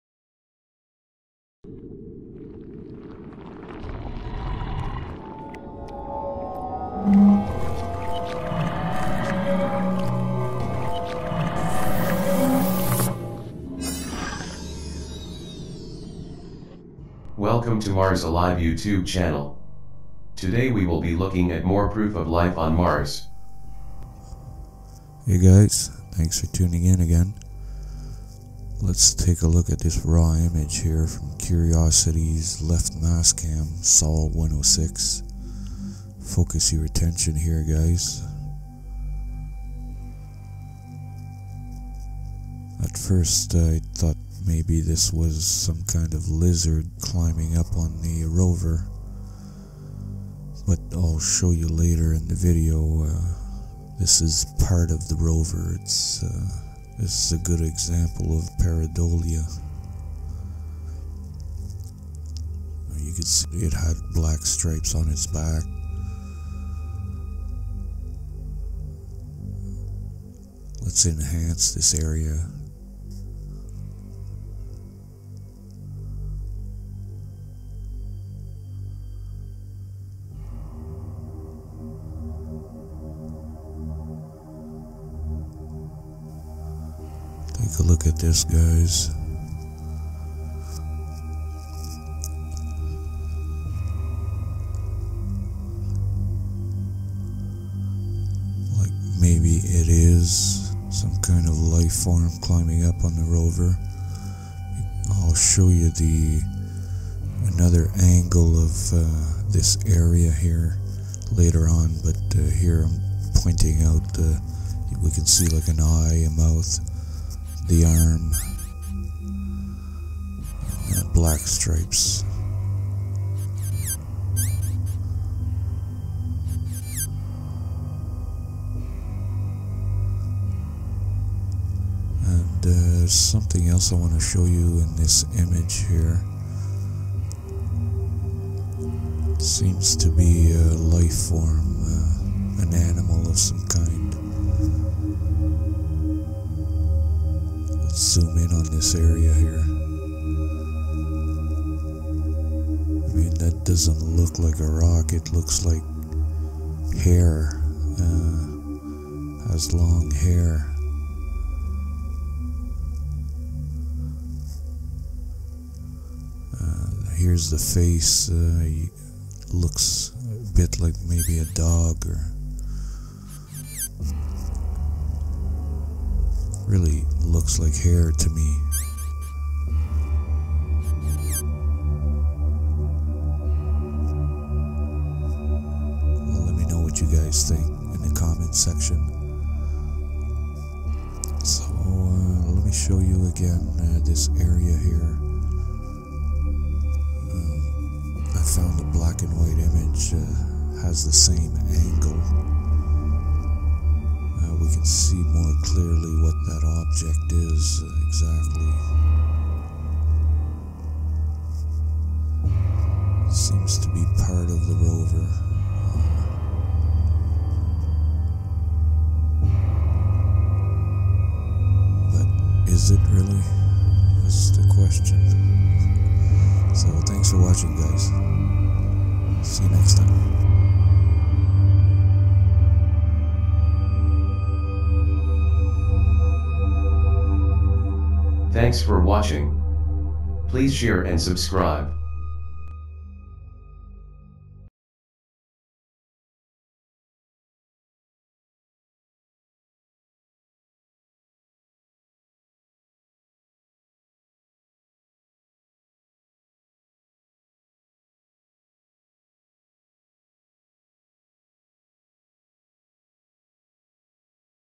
A Case Of Pareidolia on Mars....
MARS ALIVE - Searching for life on Mars!
High-Quality Video 4K Resolution!

In this image from Curiosity Rover, i show you 2 anomalies, the first one i thought looked like a lizard climbing up on the rover, but it's possible this is part of the rover, the second anomaly looks like an animal, has long hair, looks like a dog. to the left of the dog, i also noticed 2 humanoid heads, the one with dark skin is looking straight towards the rover's camera, the other has pale skin, looks like an elf with long Spock like ears.

Let me know what your thoughts are on this Mars Anomaly in the comment section below!